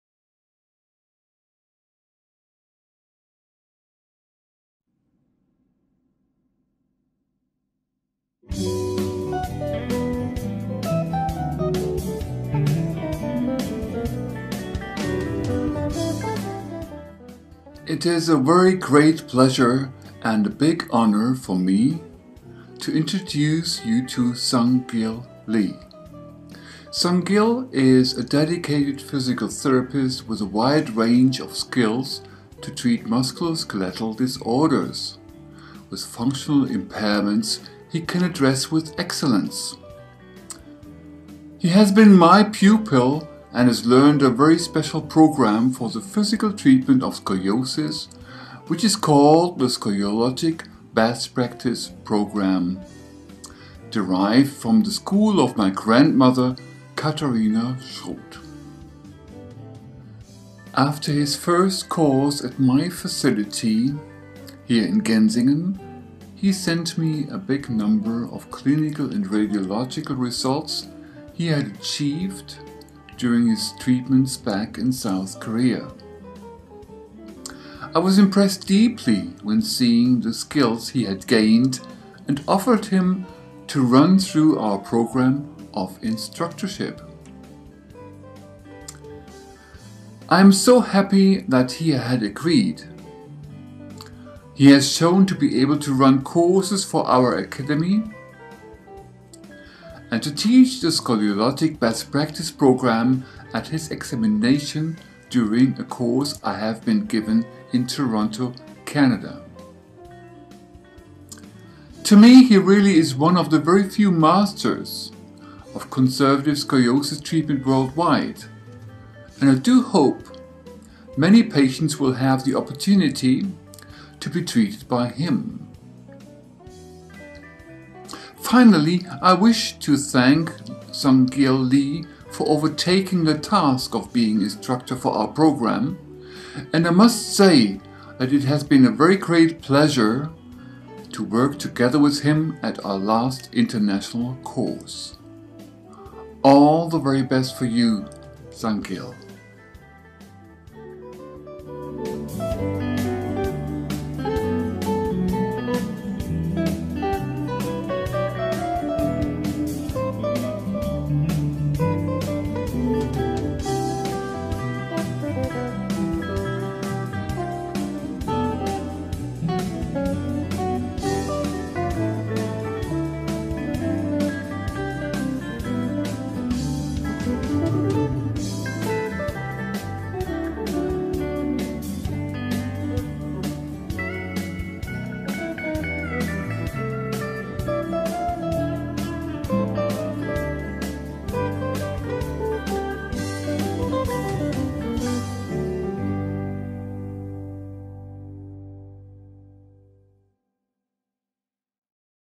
Dr. Hans Rudolf Weiss 박사님의 이상길 Schroth Master 소개입니다
Katharina Schroth 여사님, Lehnert Schroth 여사님,
그리고 3대째 이어오고 계시는 Dr. Hans Rudolf Weiss 박사님께서
K-Schroth 이상길 Schroth Master 님을 소개해주시는 내용입니다.

항상 환자분들에게 더 좋은 운동이 없을까?
어떻게 하면 환자분들이 더 열심히 운동하고 더 좋아질 수 있을까?
고민하시는 이상길 Schroth Master 입니다.